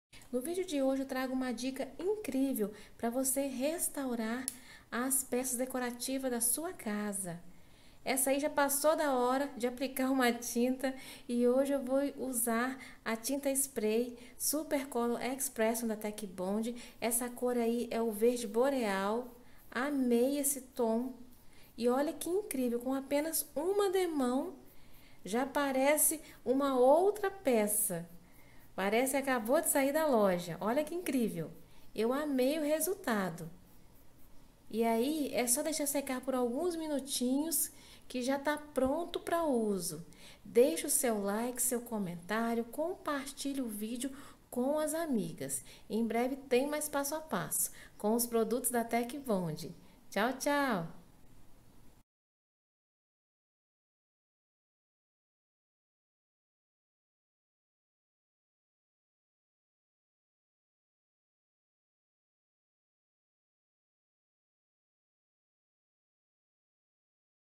Pintura fácil com Super Collor Expression da TEKBOND - Flávia Martins
Para este passo a passo utilizei:
Tinta Super Color Expression cor VERDE BOREAL